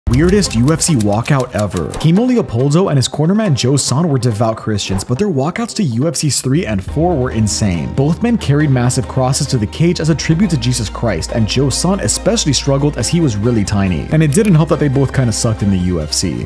STRANGEST Walkout in UFC History | Kimo Leopoldo and Joe Son Carrying Cross in UFC
"Weirdest UFC Walkout Ever

Kimo Leopoldo and his cornerman Joe Son were devout christians, but their walkouts to UFC’s 3 and 4 were insane.

Both men carried massive crosses to the cage as a tribute to Jesus Christ, and Joe Son especially struggled as he was really tiny.

And it didn’t help that they both kind of sucked in the UFC